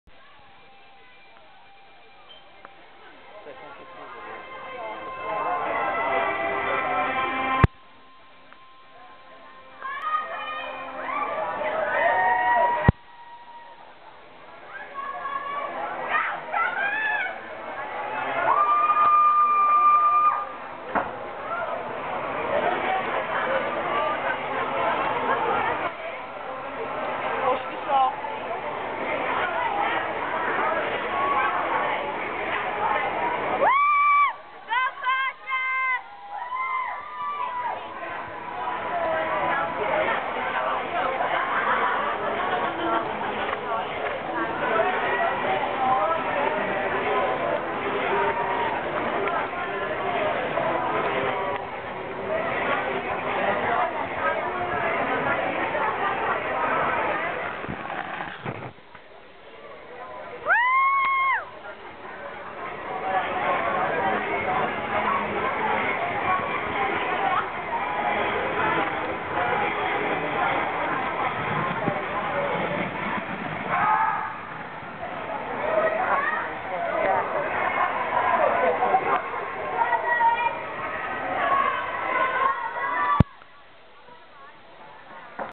me & magic evening performance
Me & Magic at the UK Showing Premier Show at Bold Heath Equestrian Centre in the final go around of the ridden welsh D in the evening performance